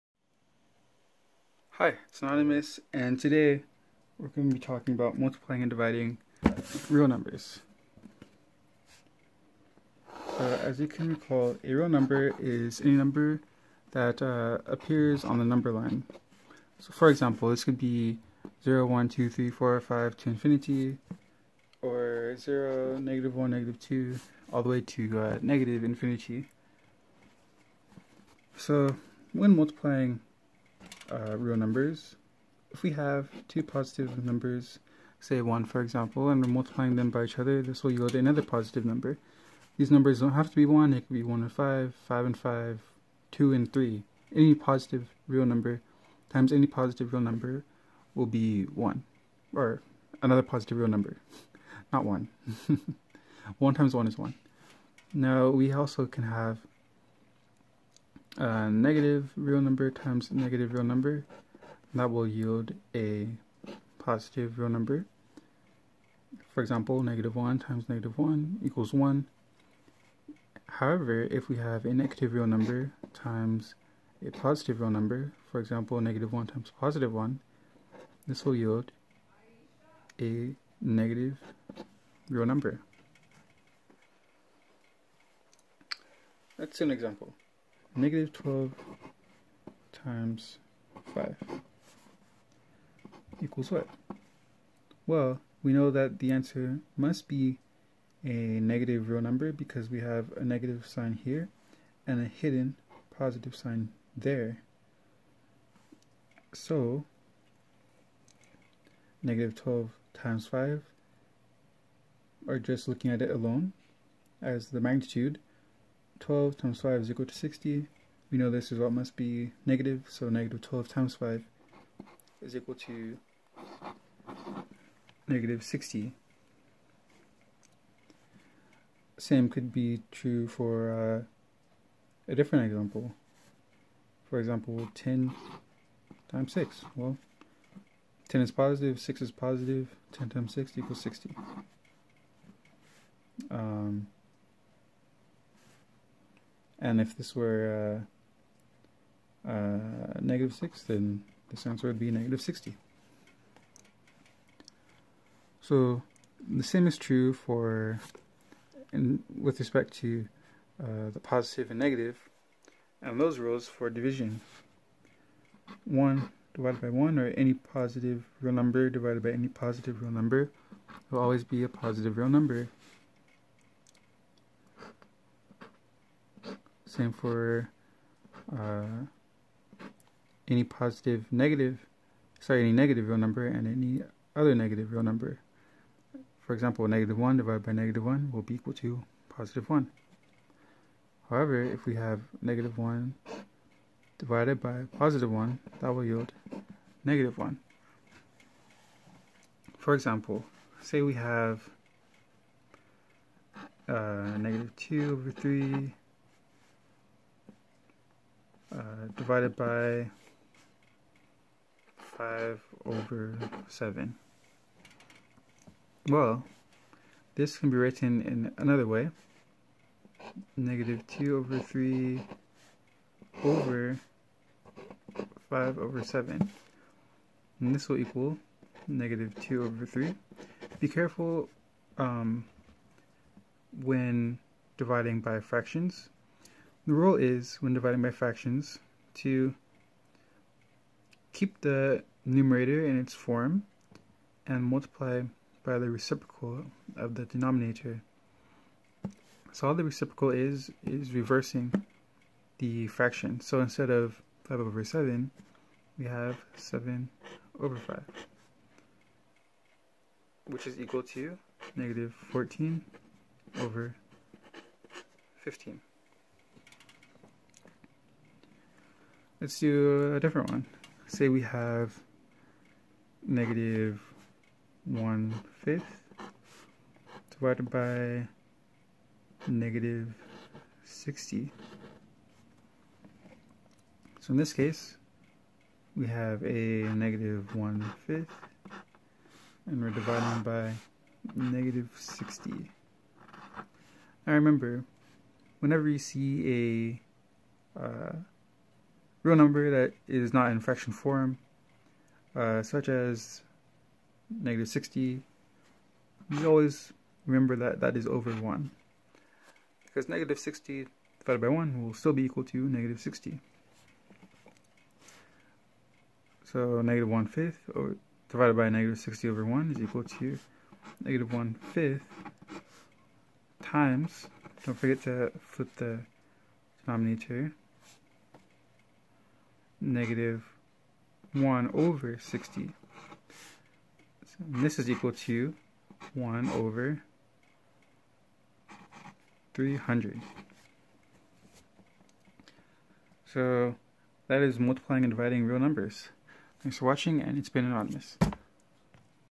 Multiplying and Dividing Real Numbers
Here, I discuss what a real number is and how to use them.